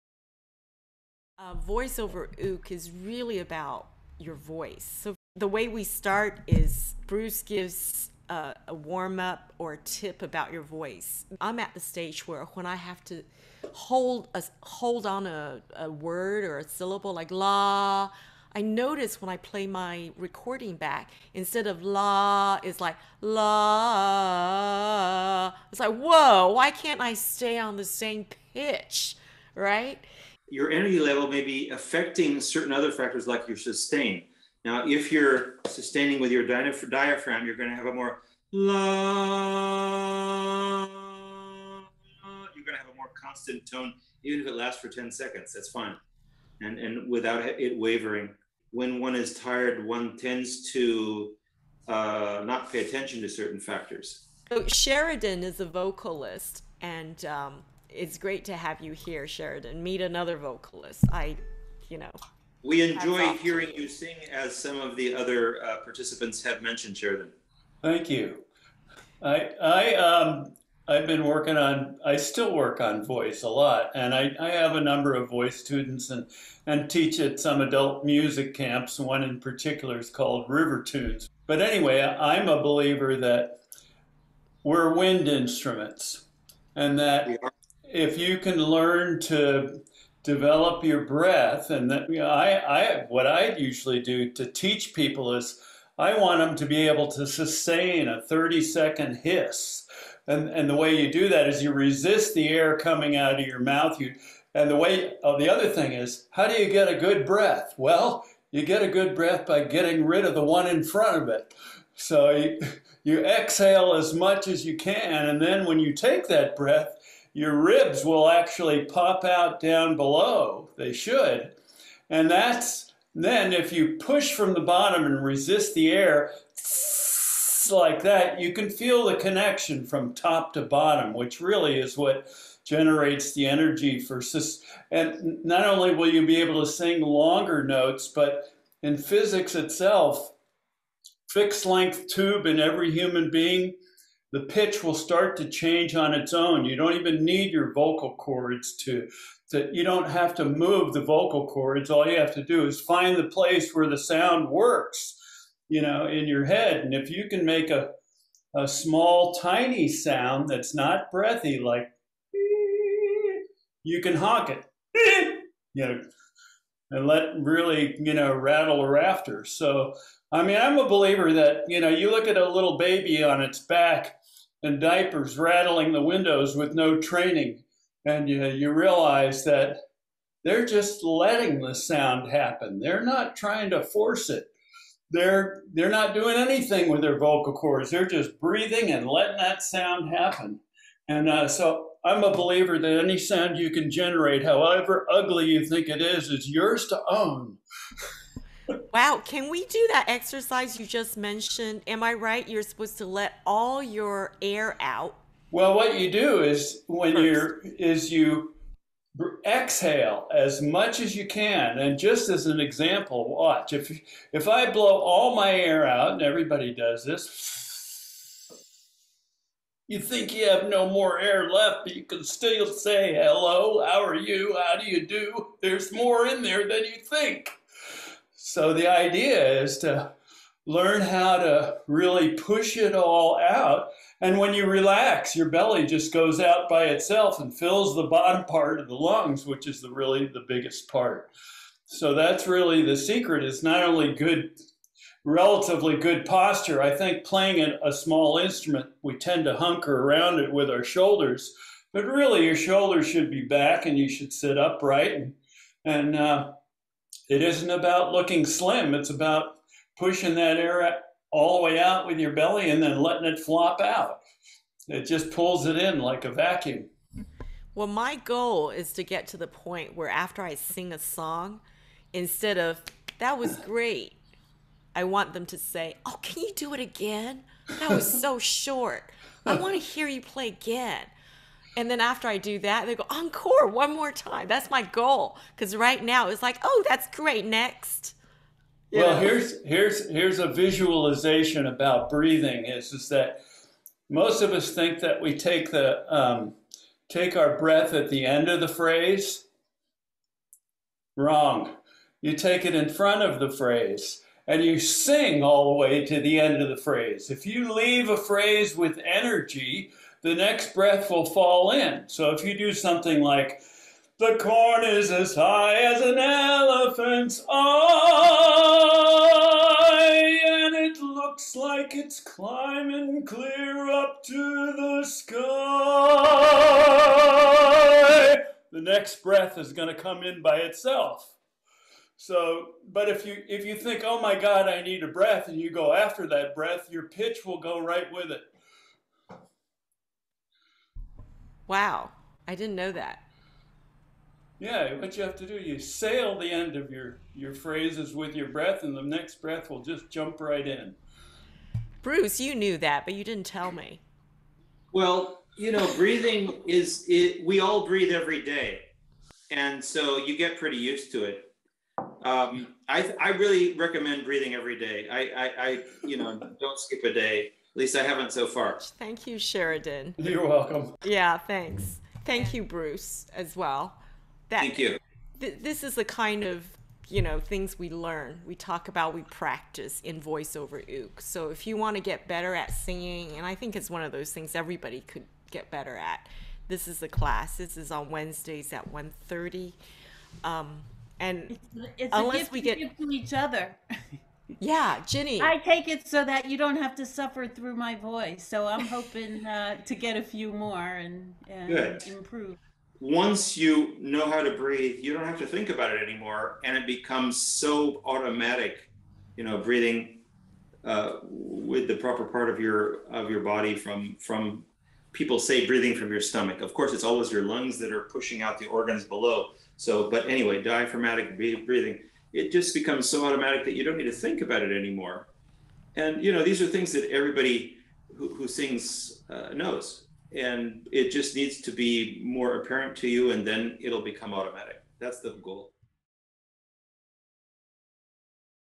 Voice Over Uke class
How do you feel about using your voice to sing? Answer these 10 questions
Receive a bespoke vocal exercise that you can play on the ukulele to help you warm up.

Voice Over Uke
8-week (1-hour) class in Zoom
Voice instructor: Bruce Boreham (Venice, Italy)
Rhythm, ukulele instructor: Anne Ku (Boston, Massachusetts)

Voice Over Uke class is starting on Wednesday 4/26/2022 at 1:30 PM EDT.
The class is capped at 10 people, so everyone gets individual voice coaching from Bruce, while everyone else is in group exercises (rhythm, harmony, etc) in with Anne.  Tuition includes class video recordings and handouts, including vocal and rhythmic exercises designed for the participants in this small class.

This video was extracted from recordings from 3 Chord Thursday Earth Day 4/21/2022